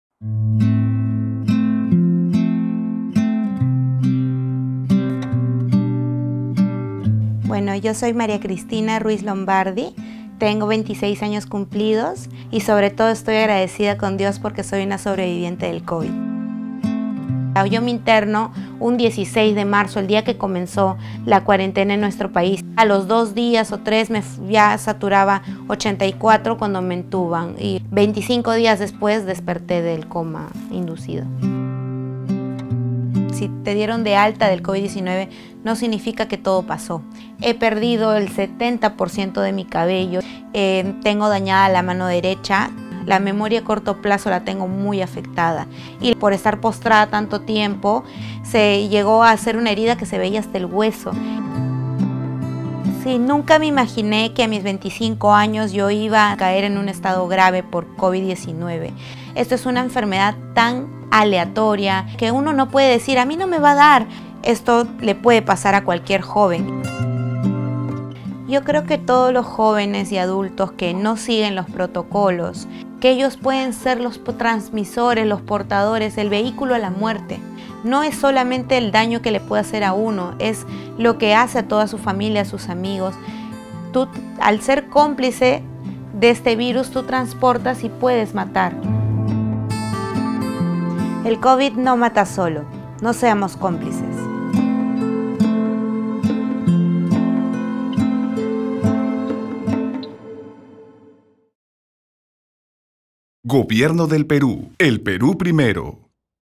NoSeamosCómplices
María Cristina Ruiz tiene 26 años y es una joven sobreviviente del COVID-19. Su lucha le dejó secuelas y aquí puedes conocer su historia de vida.